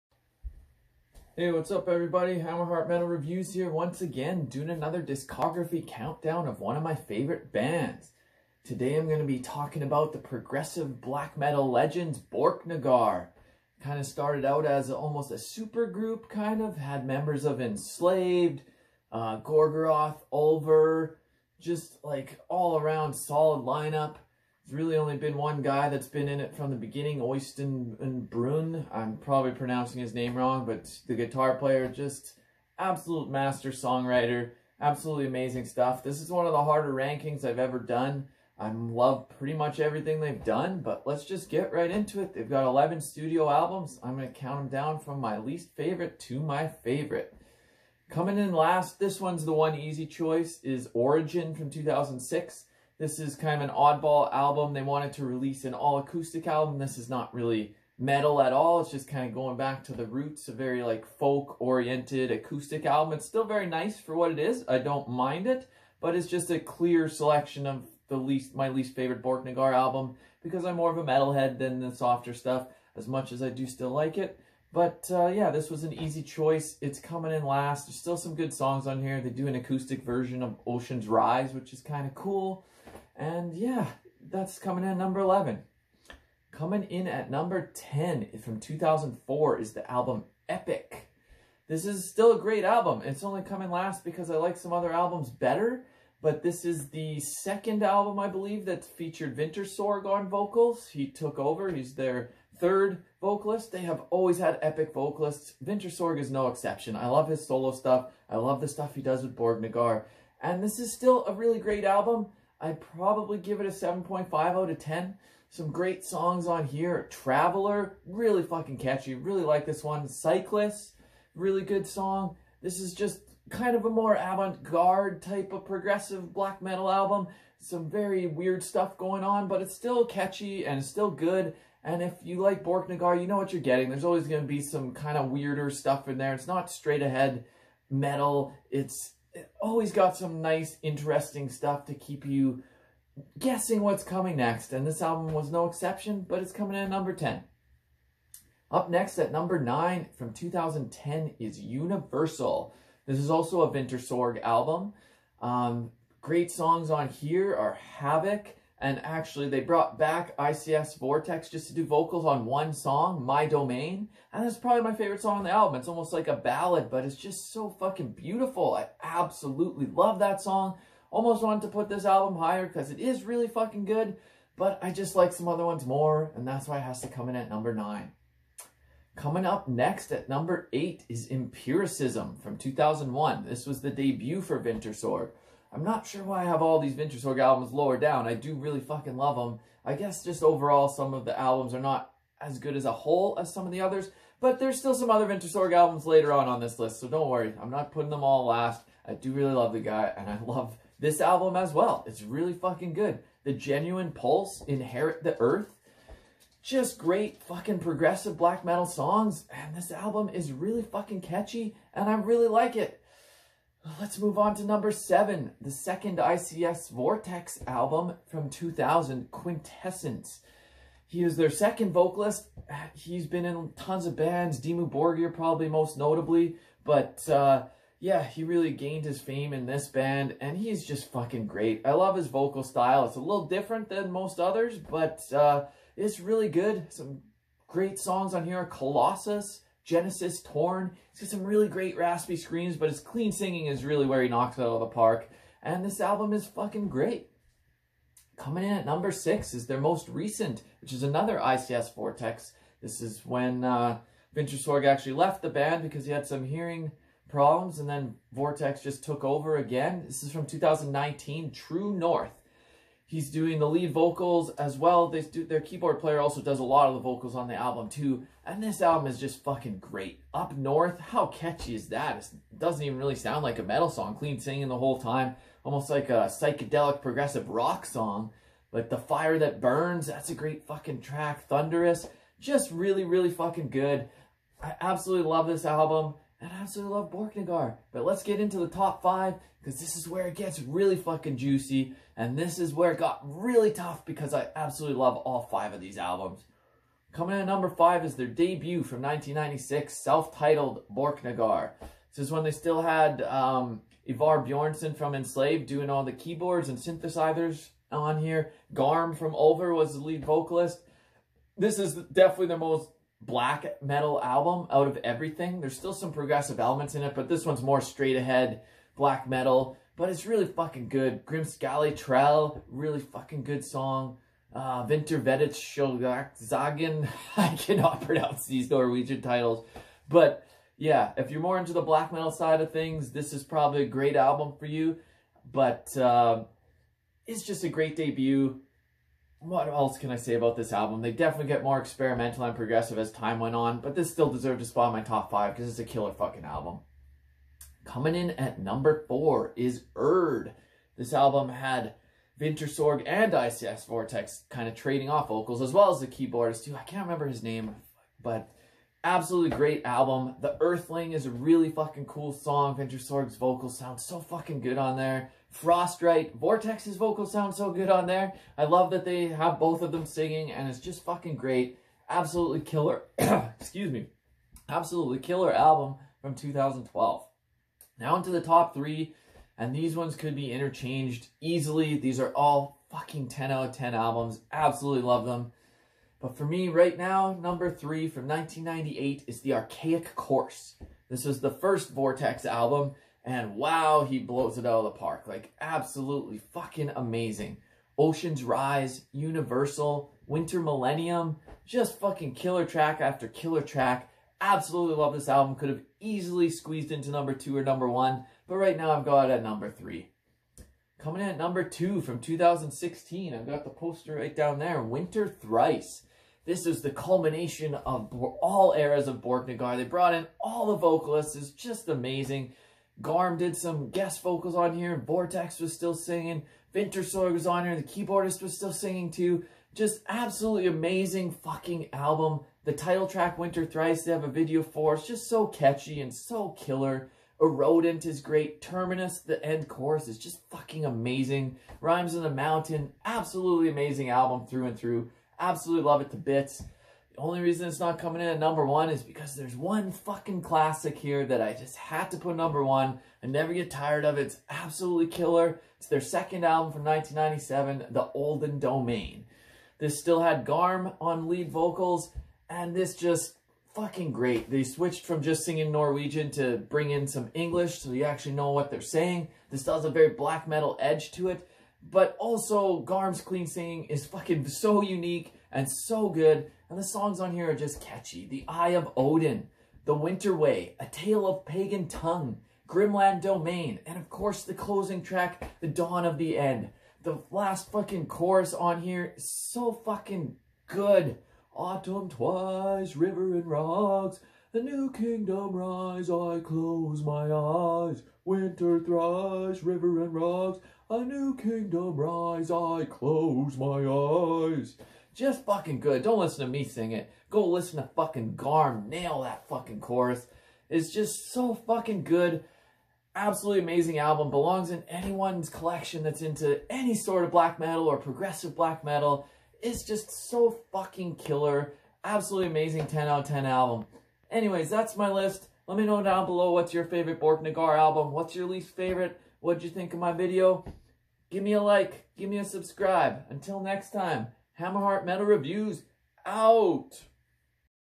Borknagar - Ranking the Studio Albums
Ranking all of Borknagar's albums from my least favorite to my favorite